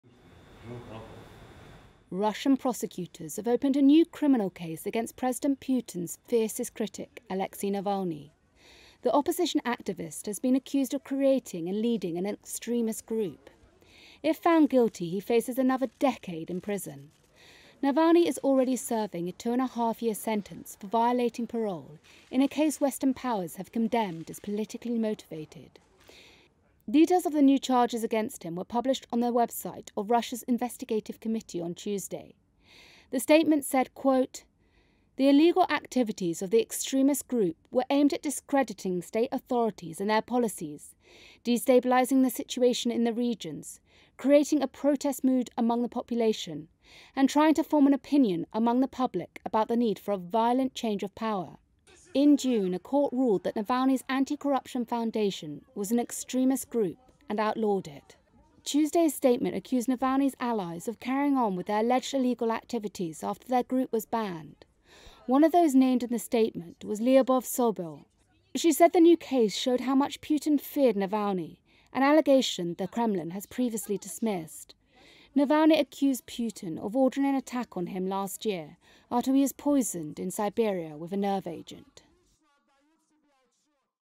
Russia opens new case against Navalny
Russia sharply escalated a campaign against jailed Kremlin critic Alexei Navalny on Tuesday, opening a new criminal case against him that could mean another decade in jail.

VIDEO: REUTERS